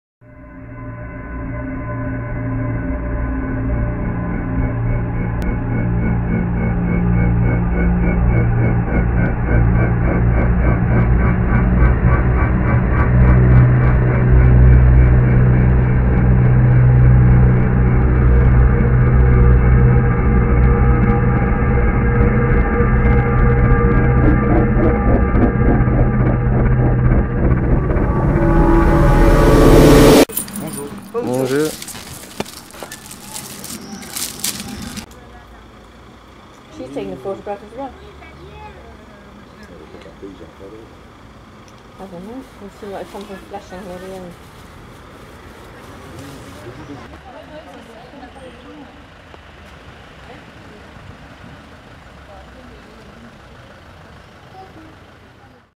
The Bees of Valbonne
Test of the Canon 60D with EF-S 18-135mm f/3.5-5.6 Is Lens in Valbonne, in the Provence-Alpes-Côte d'Azur. A most terrible and heinous plague befalls the town of Valbonne. A beehive has exploded, expelling plumes of angry bees in all directions. The police and fire services respond rapidly, safely cordoning off the bees and taking photographs to show their friends. All footage is raw.